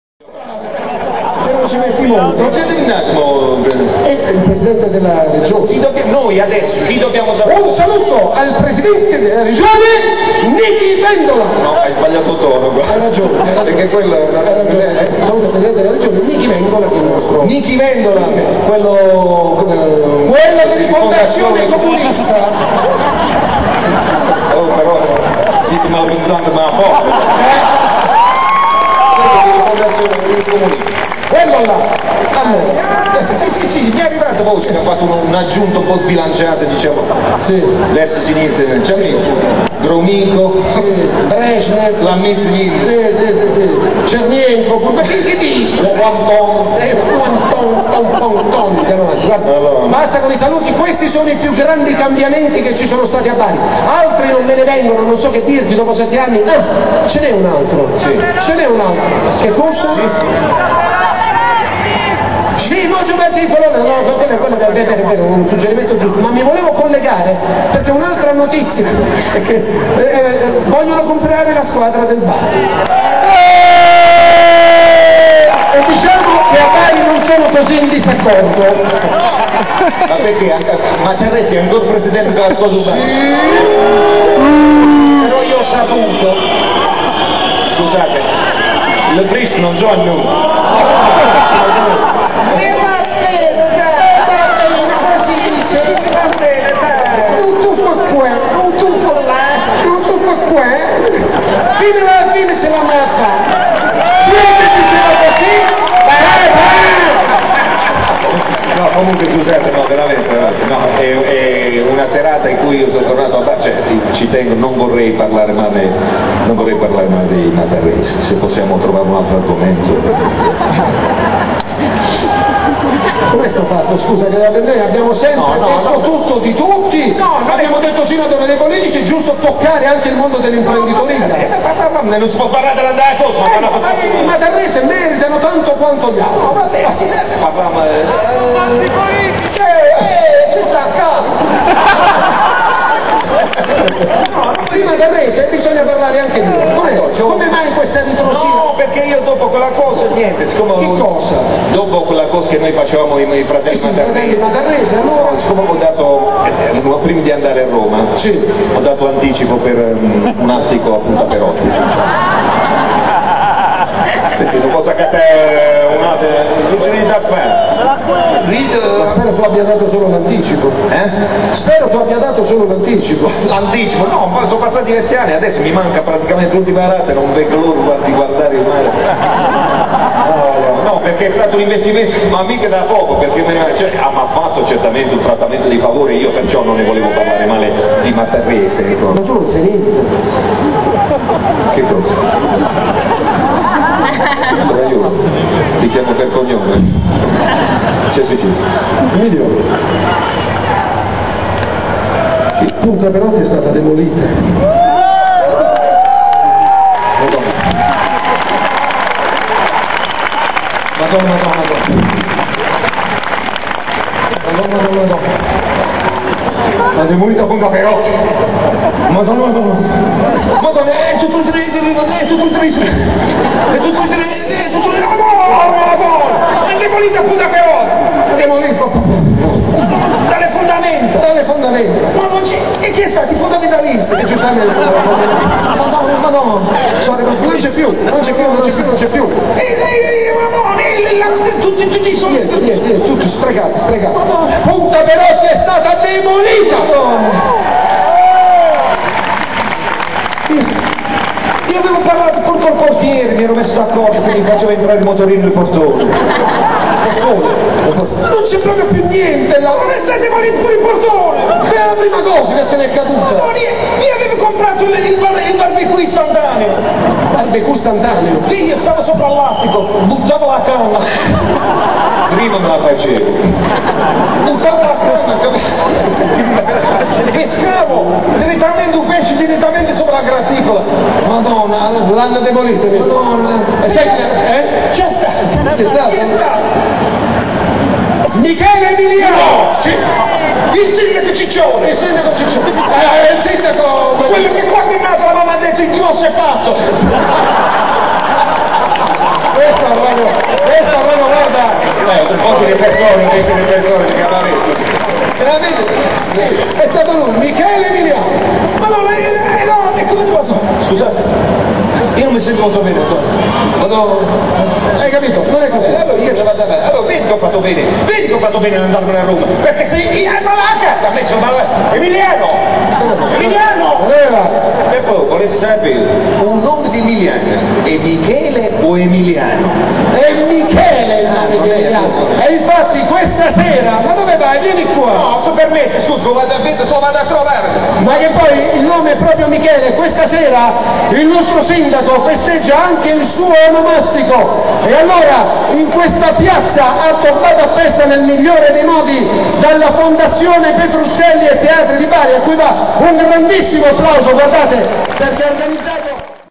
Toti e Tata, il ritorno (pt.3) - Bari 29-09-2007
Emilio Solfrizzi e Antonio Stornaiolo di nuovo insieme a Bari per la "notte metropolitana dei castelli" salutano le istituzioni... (GRANDISSIMI)